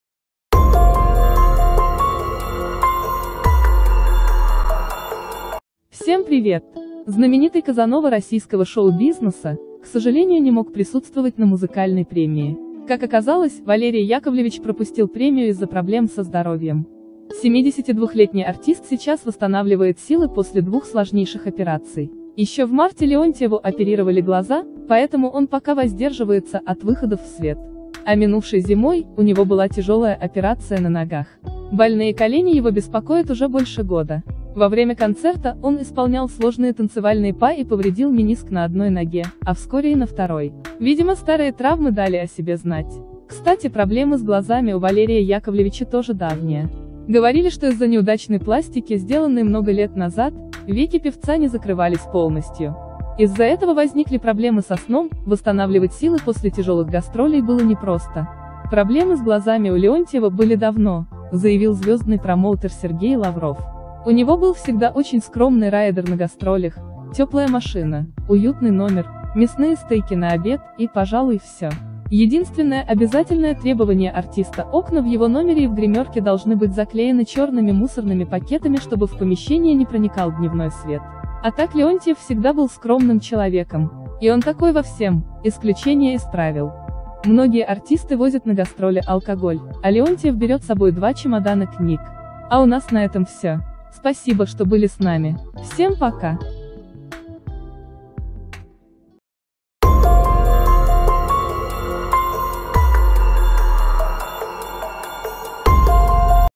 Не восстановить! Промоутер Леонтьева заявил о тяжелом состоянии артиста.
Валерий Леонтьев перестал выходить на связь.